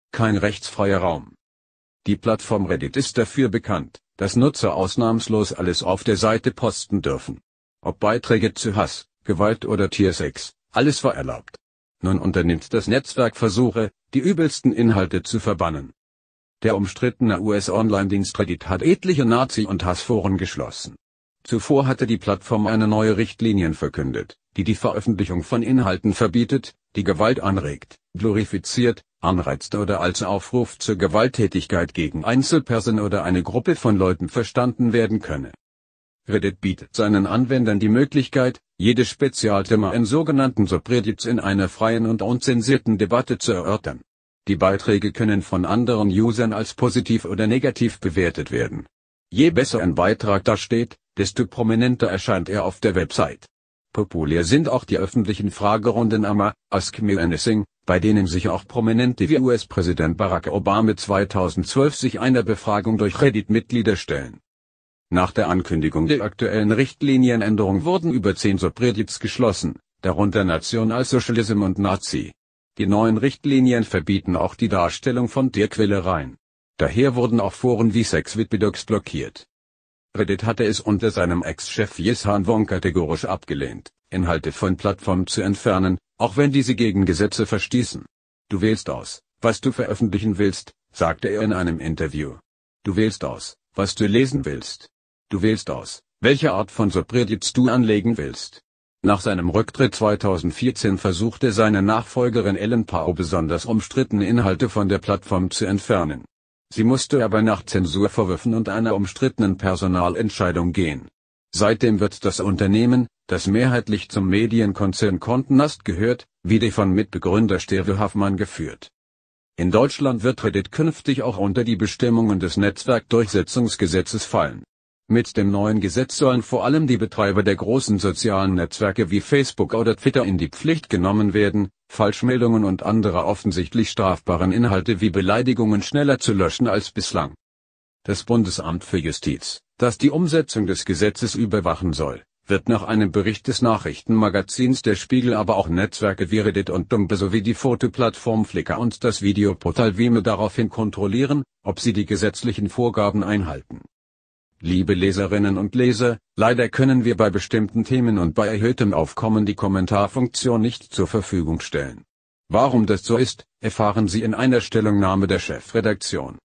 Reddit schließt etliche Nazi- und Tiersex-Foren
Kein rechtsfreier Raum
Die Plattform Reddit ist dafür bekannt, dass Nutzer ausnahmslos alles auf der Seite posten dürfen. Ob Beiträge zu Hass, Gewalt oder Tiersex – alles war erlaubt. Nun unternimmt das Netzwerk Versuche, die übelsten Inhalte zu verbannen.
Der umstrittene US-Onlinedienst Reddit hat etliche Nazi- und Hass-Foren geschlossen.